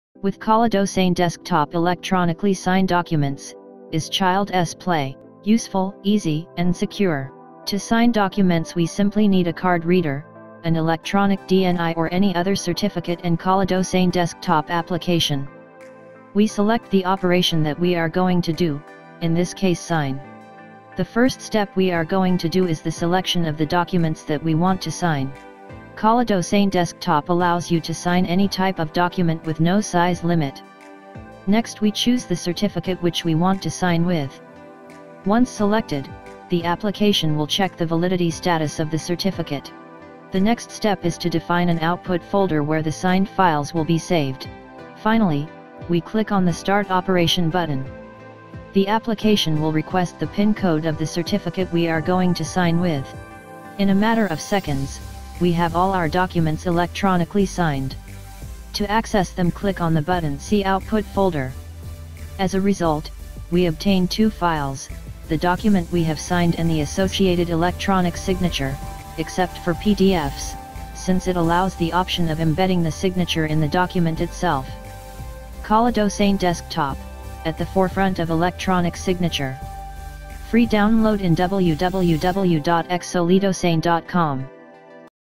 Sign documents with XolidoSign Desktop
Program FREE for signing, verifying and time-stamping documents on your computer.